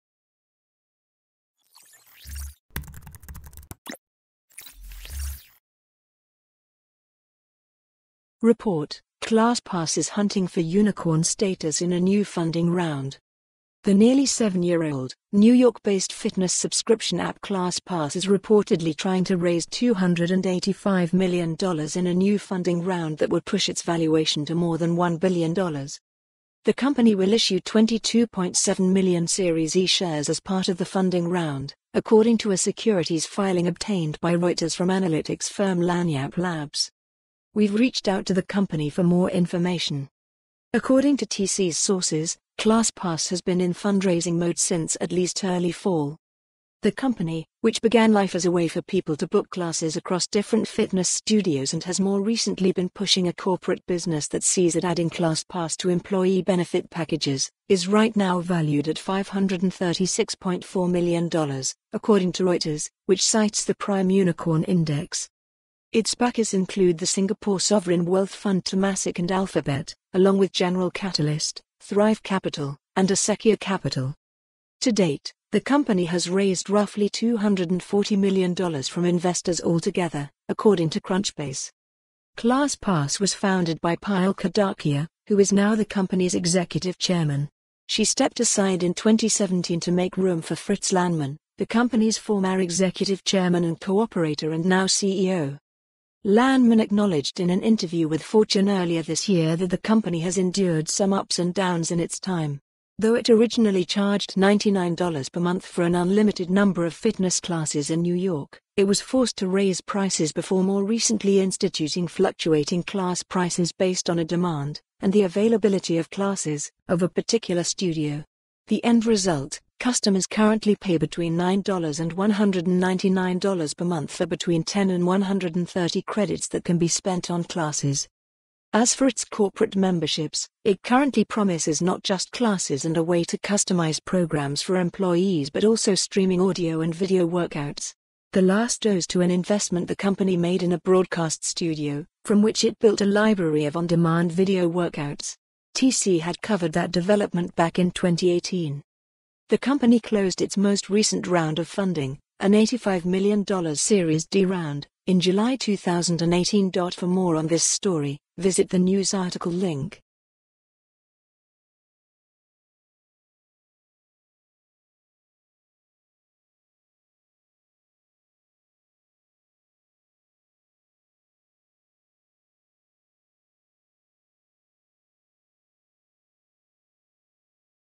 Report: ClassPass is hunting for unicorn status in a new funding round
The nearly seven-year-old, New York-based fitness subscription app ClassPass is reportedly trying to raise $285 million in a new funding round that would push its valuation to more than $1 billion.

The company will issue 22.7 million Series E shares as part of the funding round, according to a securities filing obtained by Reuters from analytics firm Lagniappe Labs.

We've reached out to the company for more information.

According to TC's sources, ClassPass has been in fundraising mode since at least early fall.

The company - which began life as a way for people to book classes across different fitness studios and has more recently been pushing a corporate business that sees it adding ClassPass to employee benefit packages - is right now valued at $536.4 million, according to Reuters, which cites the Prime Unicorn Index.

Its backers include the Singapore sovereign wealth fund Temasek and Alphabet, along with General Catalyst, Thrive Capital, and Acequia Capital.

To date, the company has raised roughly $240 million from investors altogether, according to Crunchbase.

ClassPass was founded by Payal Kadakia, who is now the company's executive chairman. She stepped aside in 2017 to make room for Fritz Lanman, the company's former executive chairman and co-operator and now CEO.

Lanman acknowledged in an interview with Fortune earlier this year that the company has endured some ups and downs in its time. Though it originally charged $99 per month for an unlimited number of fitness classes in New York, it was forced to raise prices before more recently instituting fluctuating class prices based on a demand (and the availability of classes) of a particular studio.  The end res